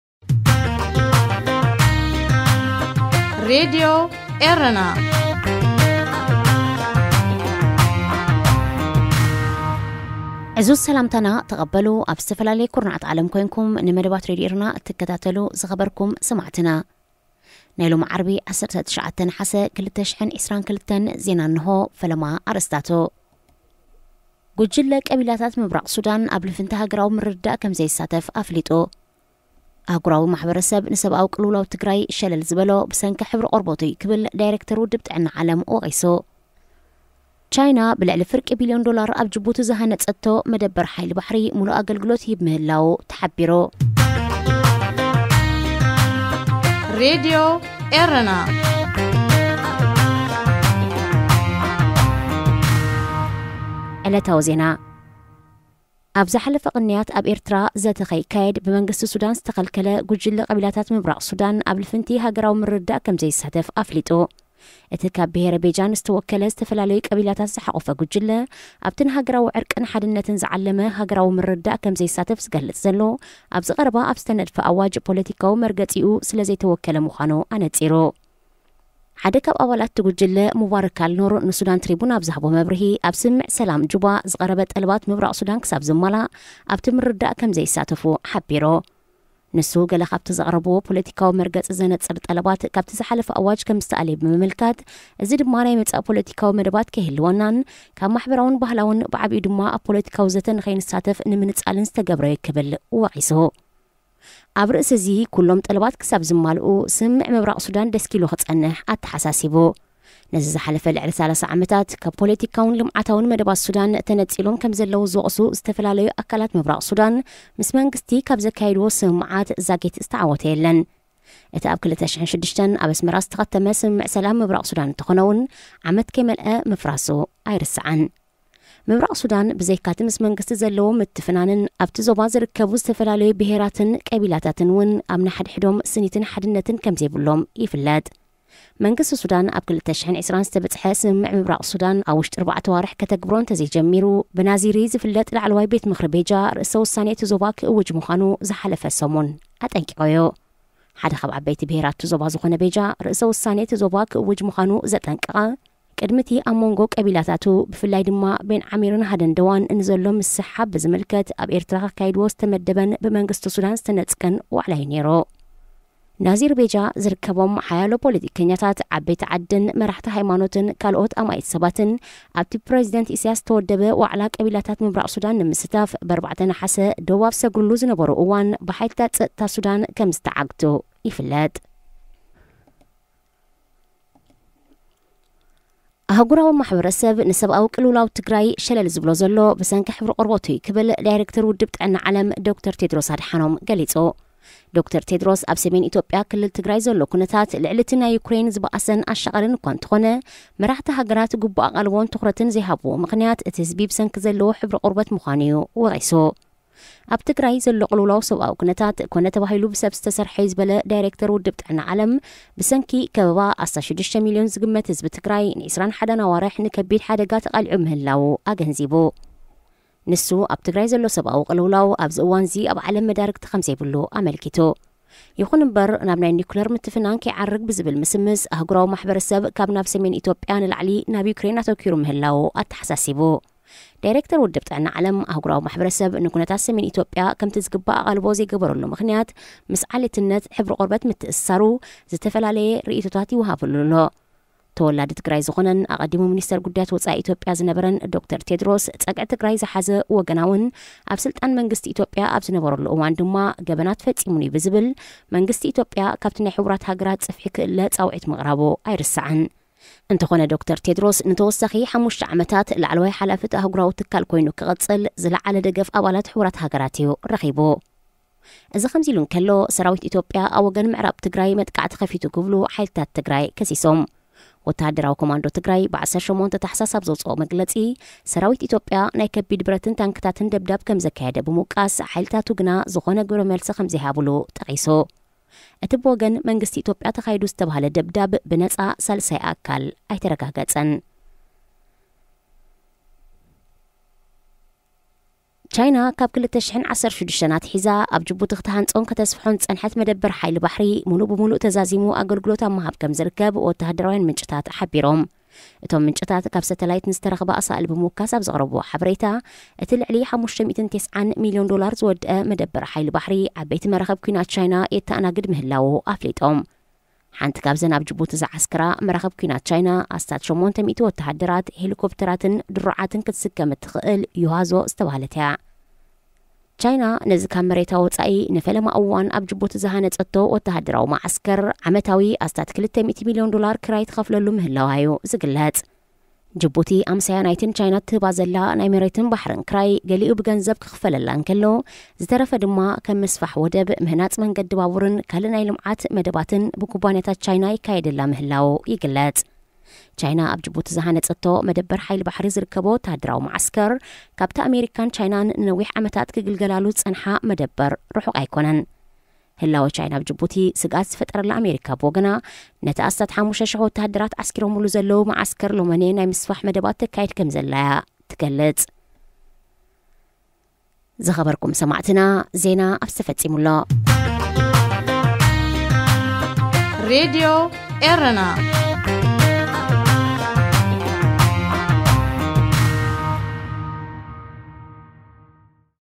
Daily News:- Eritrea, Eastern Sudan, Ethiopia, Djibouti, China. Friday, 19 August 2022.
ዕለታዊ ዜና - ዓርቢ 19 ነሓሰ 2022.
     -  ጉጅለ ቀቢላታት ምብራቕ ሱዳን፣ ኣብ ልፍንቲ ሃገራዊ ምርድዳእ ከምዘይሳተፍ ኣፍሊጡ።
     -  ‘ ኣህጉራዊ ማሕበረሰብ፣ ንሰብኣዊ ቅልውላው ትግራይ ሸልል ዝበሎ፣ ብሰንኪ ሕብሪ ቆርበቱ’ዩ።’ ዳይረክተር ውድብ ጥዕና ዓለም
     -  ቻይና፣ ብልዕሊ ፍርቂ ቢልዮን ዶላር ኣብ ጂቡቲ ዝሃነጸቶ መደበር ሓይሊ ባሕሪ ምሉእ ኣገልግሎት ይህብ ምህላዉ ተሓቢሩ።